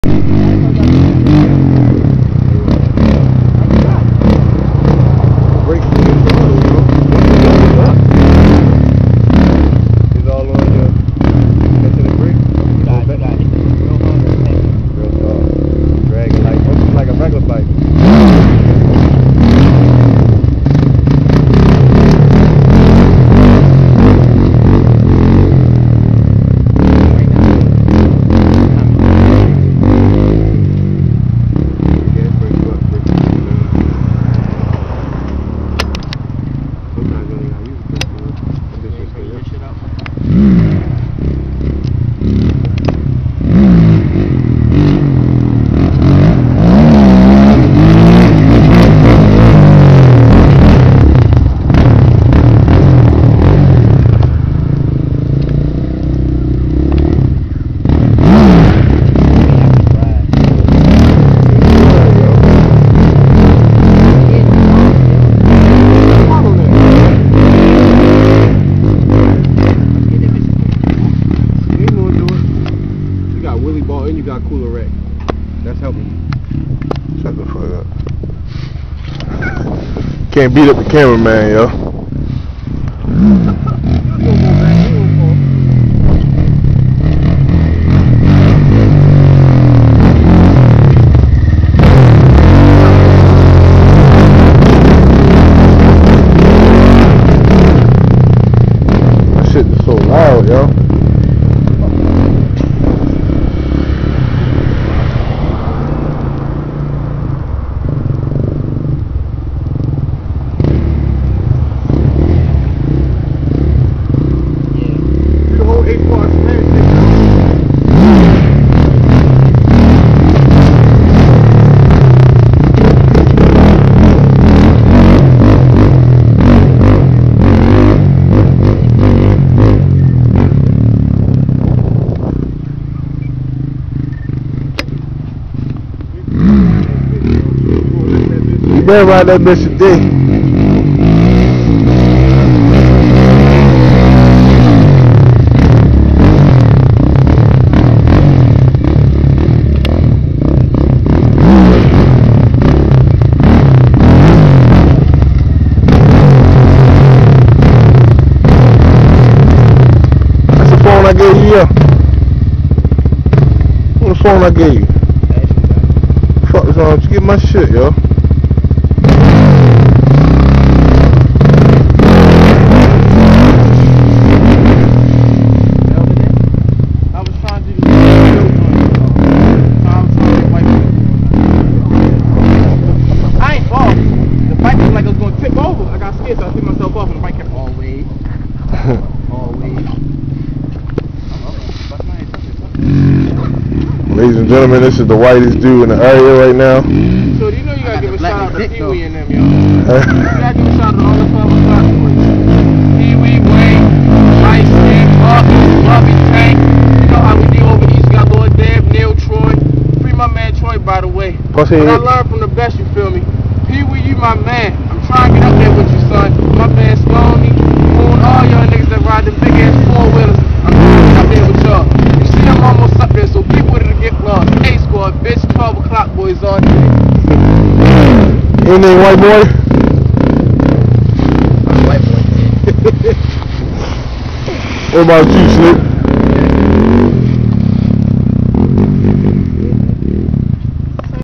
baltimore dirt bike ridin
ace squad on the big suzuki z400